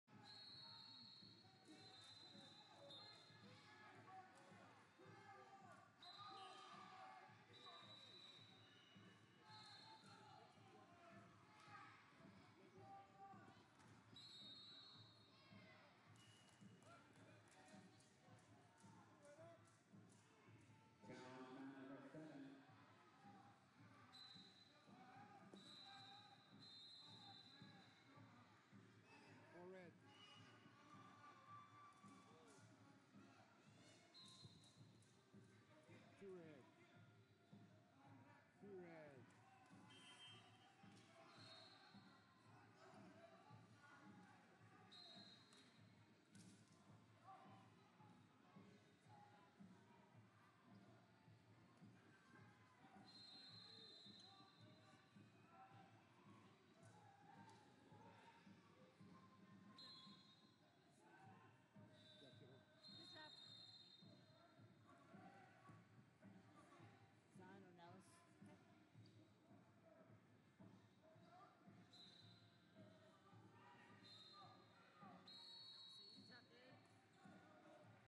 97 Lbs Champ. Round 2 - Carter Shin, VA Vs Jackson Rugaard, CO 586f
2022 Kids Freestyle & Greco-Roman National Championships - 97 Lbs Champ. Round 2 - Carter Shin, VA Vs Jackson Rugaard, CO 586f